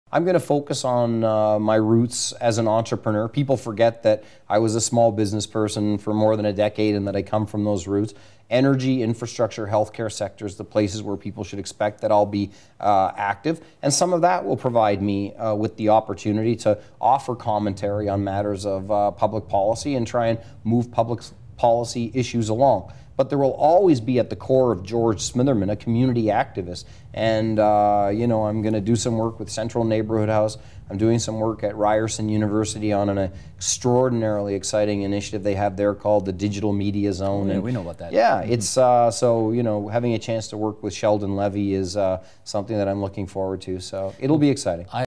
George Smitherman: What's Next?
George Smitherman's post-political plan.  Does it sound much different to you?